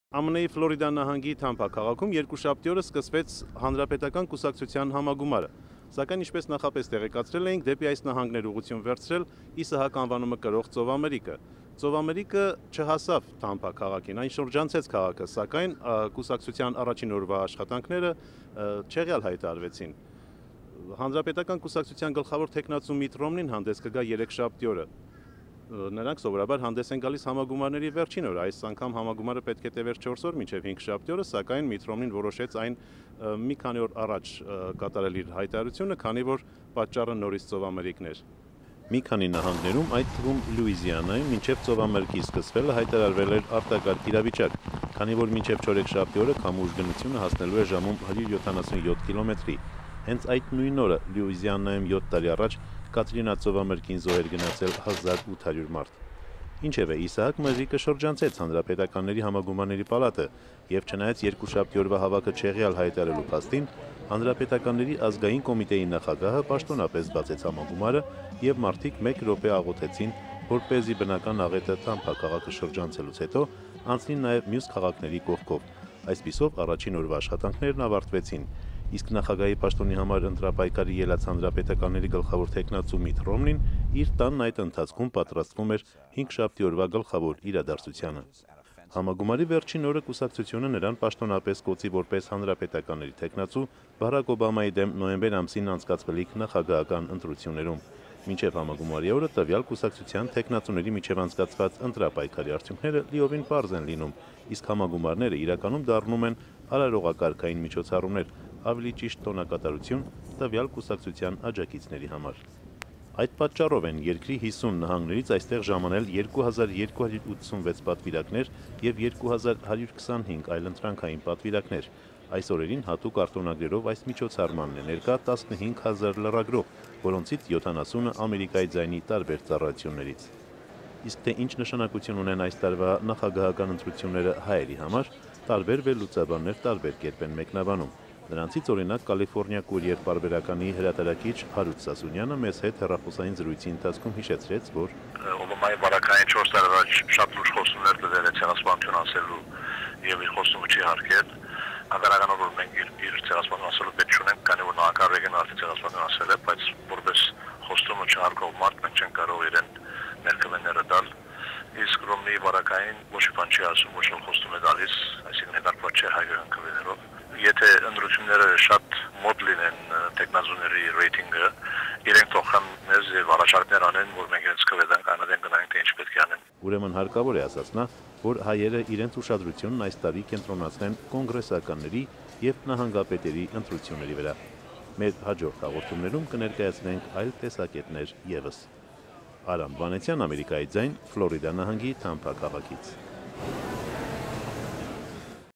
ԱՄՆ-ի հանրապետականների համագումարի 1-ին օրը
Մրրիկի պատճառով առաջին օրվա հավաքը չեղյալ հայտարարվեց